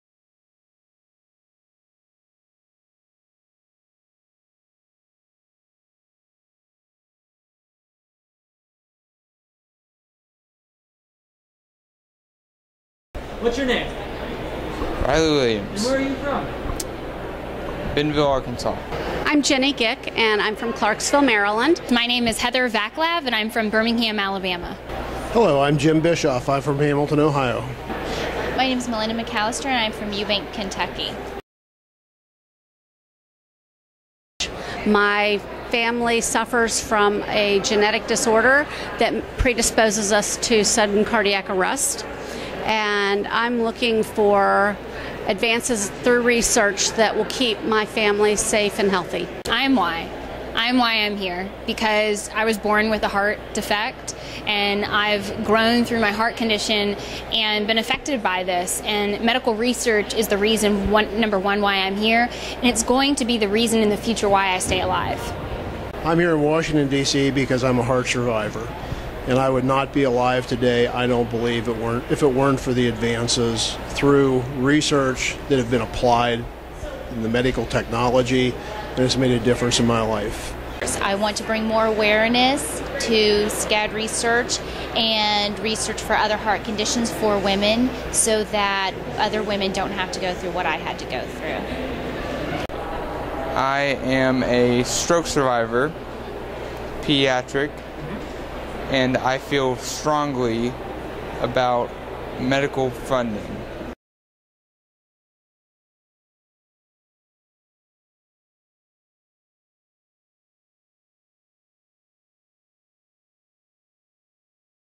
Rally for Medical Research - Advocates in their own words
On September 17th and 18th, You're the Cure advocates from across the country traveled to Washington, D.C. to meet with their lawmakers about heart and stroke research. Hear what motivates them to be an advocate.